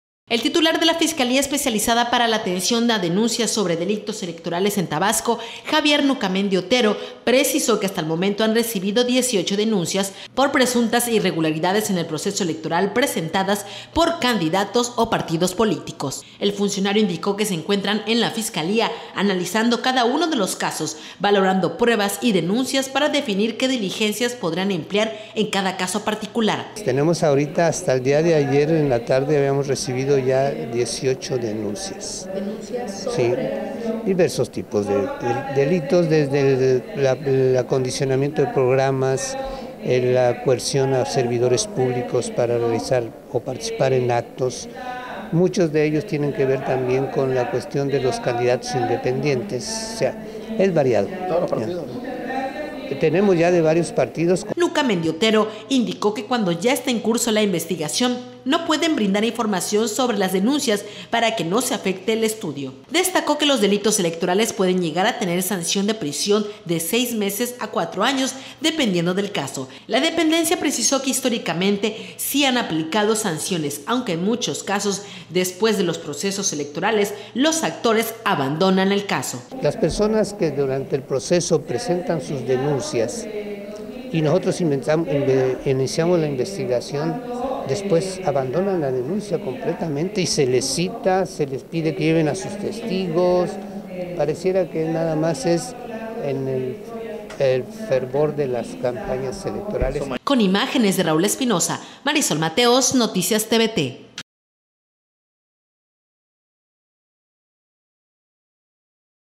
DENUNCIAN IRREGULARIDADES EN PROCESO ELECTORAL
El Titular de la Fiscalía Especializada para la Atención a Denuncias sobre Delitos Electorales en Tabasco, Javier Nucamendi, precisó que hasta el momento han recibido 18 denuncias por presuntas irregularidades en el proceso electoral